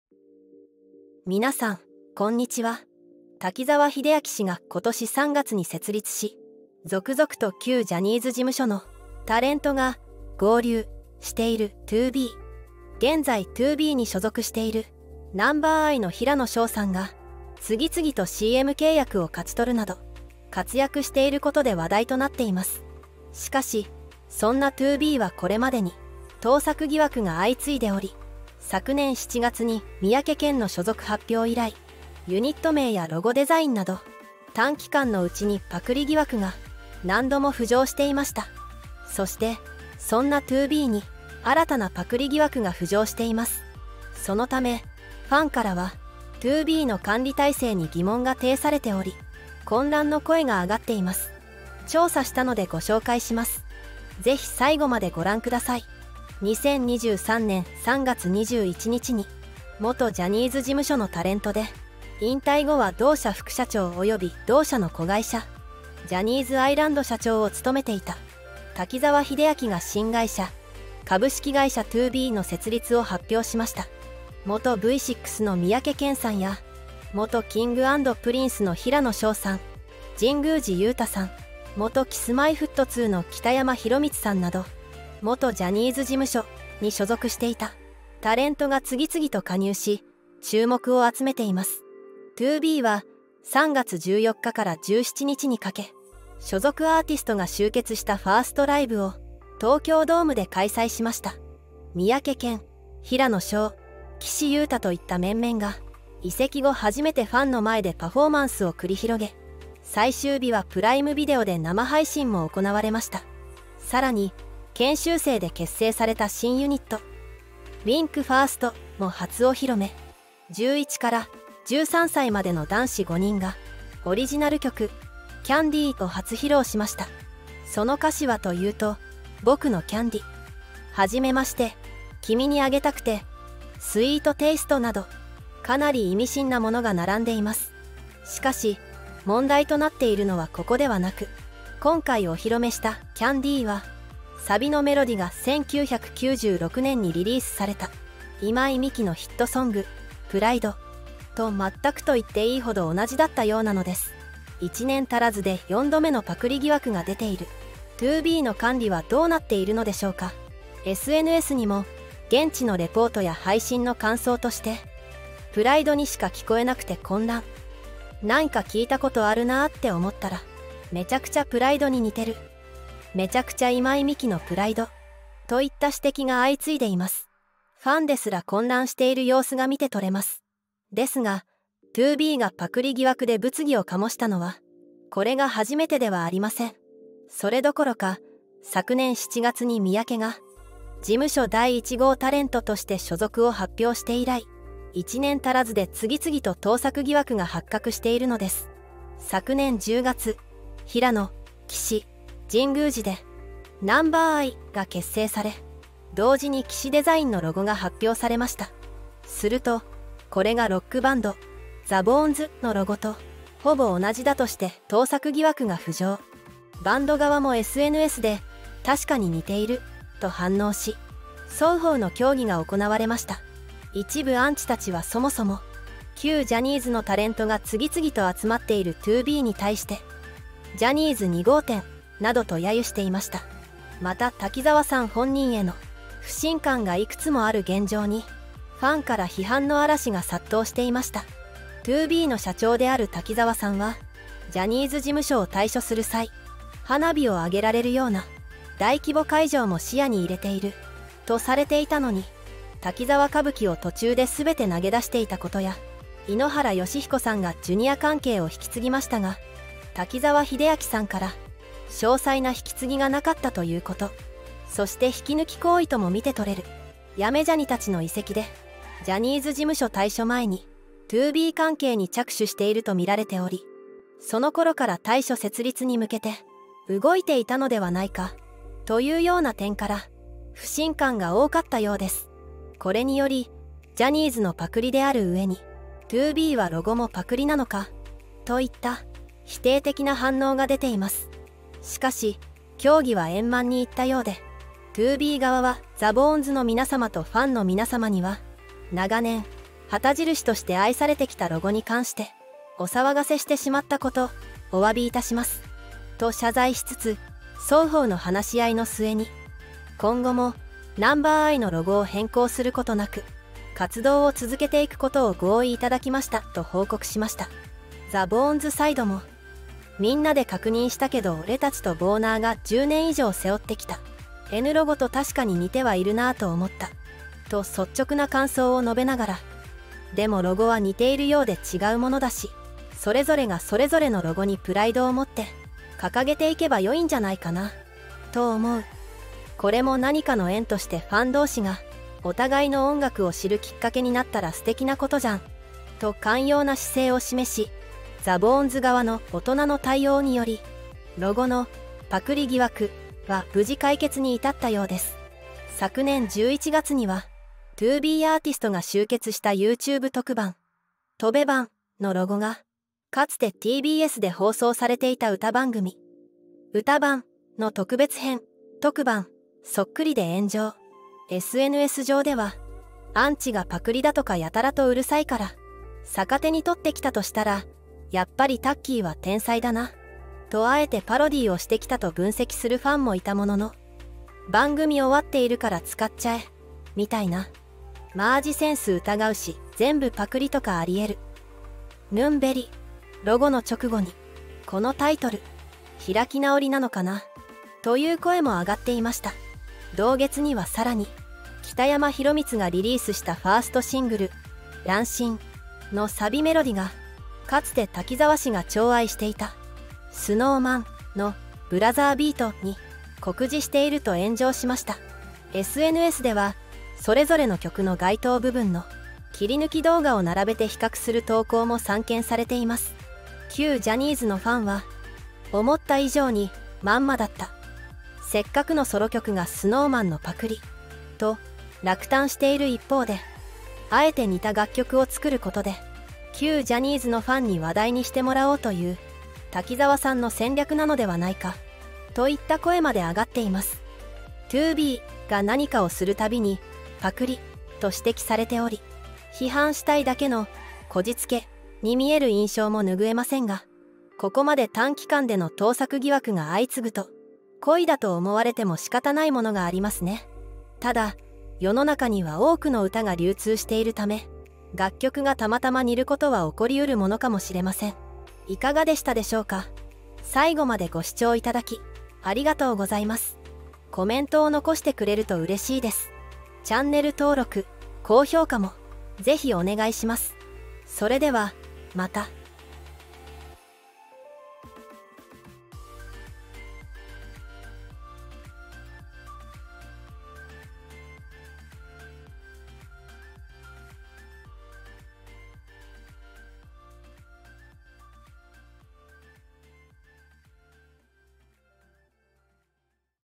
TOBEが意図的に”盗作”を続けていると言われる理由に絶句…旧事務所と決別し平野紫耀らNumber_iで一旗あげた滝沢の脅威のメディア戦略、盗作疑惑が指摘され続ける真相が…【芸能】
『日刊芸能タックル』から芸能時事をお伝えします。

滝沢秀明氏が今年3月に設立し、

続々と旧ジャニーズ事務所の

タレントが“合流”している「TOBE」。

現在TOBEに所属している

Number_iの平野紫耀さんが

次々とCM契約を勝ち取るなど

活躍していることで話題となっています。

しかし、そんなTOBEはこれまでに

盗作疑惑が相次いでおり、

昨年7月に三宅健の所属発表以来、

ユニット名やロゴデザインなど、

短期間のうちにパクリ疑惑が

何度も浮上していました。

そして、そんなTOBEに

新たなパクリ疑惑が浮上しています。

そのため、ファンからは

TOBEの管理体制に疑問が呈されており、

混乱の声が上がっています。

調査したのでご紹介します。